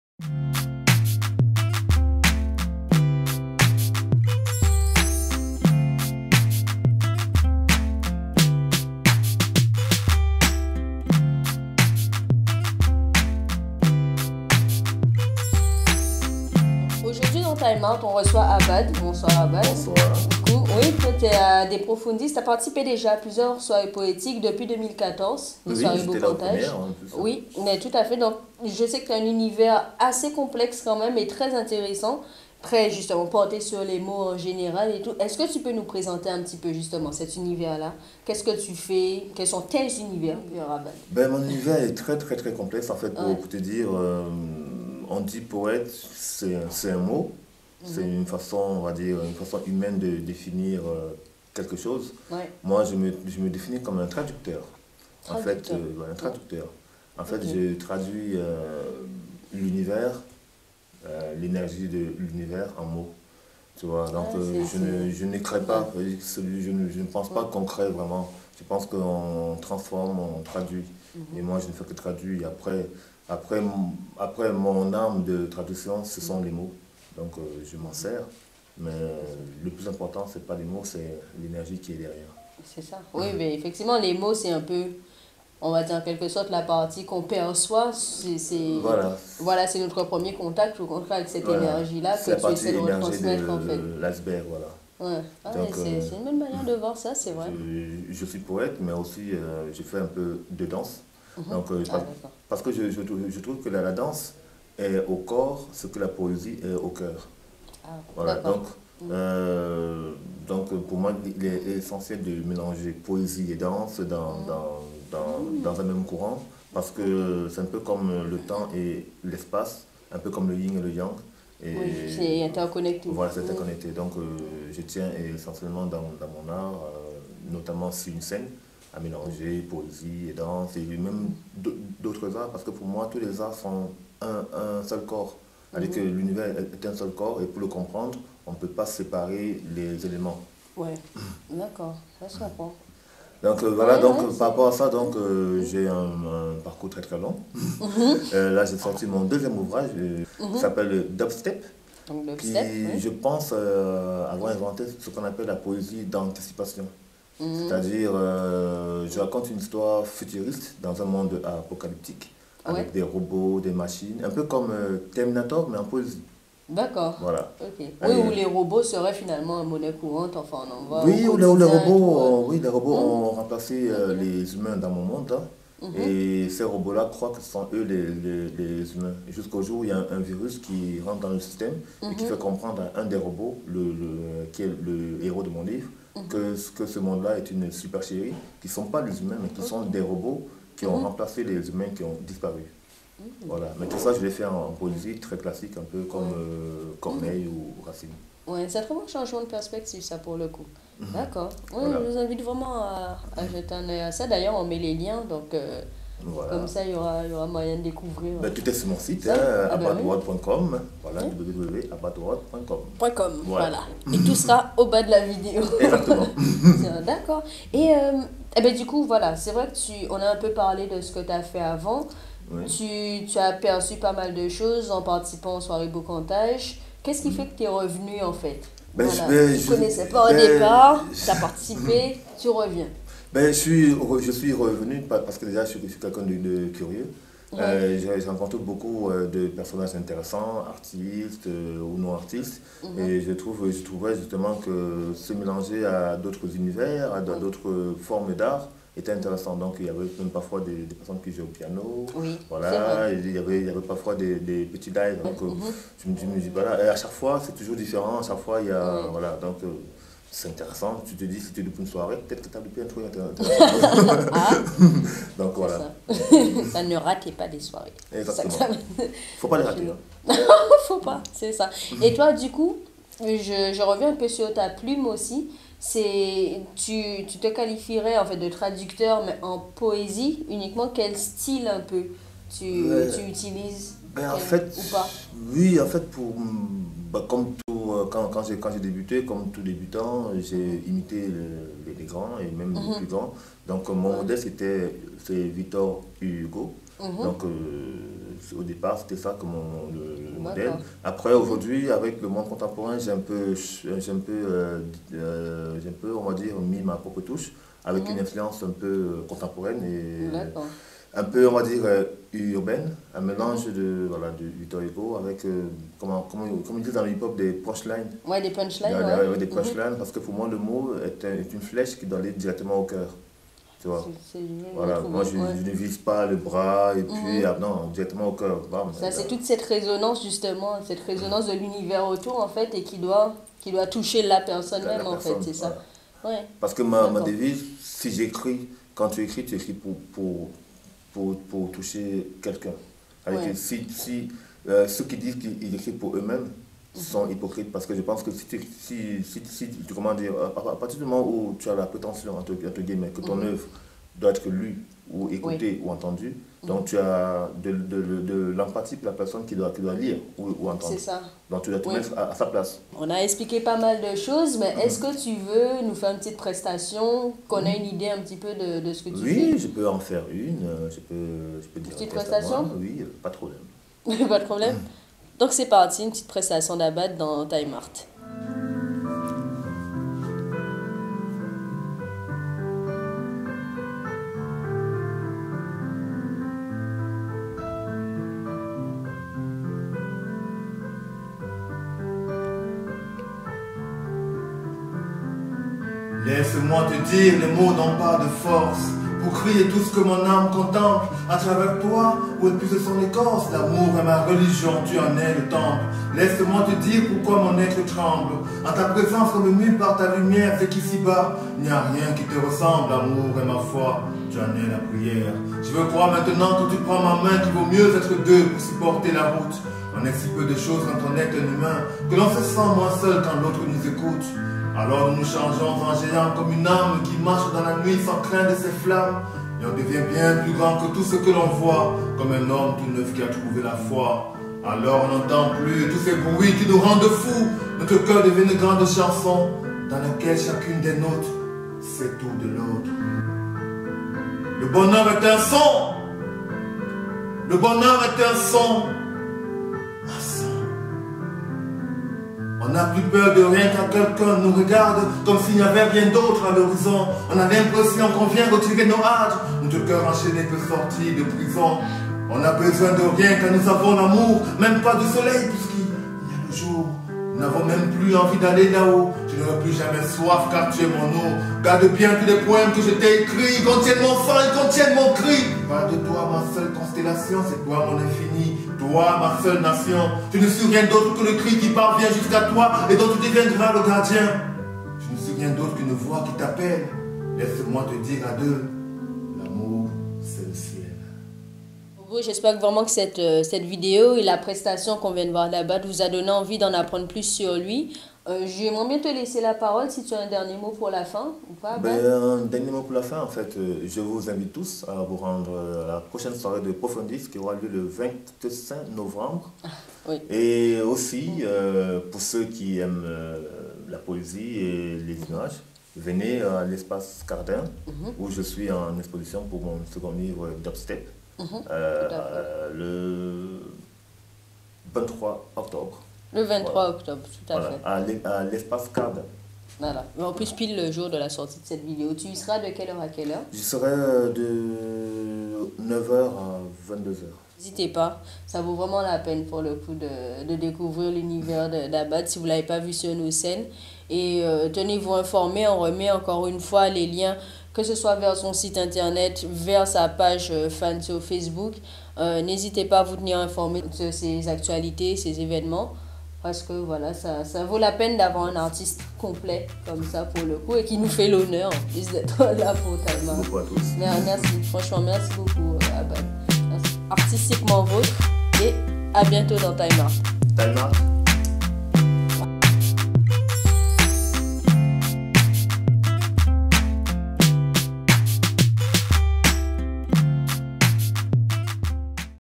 Abad Boumsong - Time Art - RPAMC - De Profundis (English subtitles + Sous-titres français)
TIME ART

Cette vidéo est consacrée à Abad.
Conscient de l'interconnexion qui relie tous les éléments de notre monde, il privilégie le pluri-artistique.
Abad est un traducteur pluriel : il manie la poésie, la danse et d'autres formes artistiques pour traduire la complexité et l'énergie créatrice de notre réalité.
This video is dedicated to Abad.
Being aware that everything is related in our world, he creates pluri-artistic works.
Abad is a resourceful translater: he uses poetry, dance and other arts to translate the complexity and the creative energy of our reality.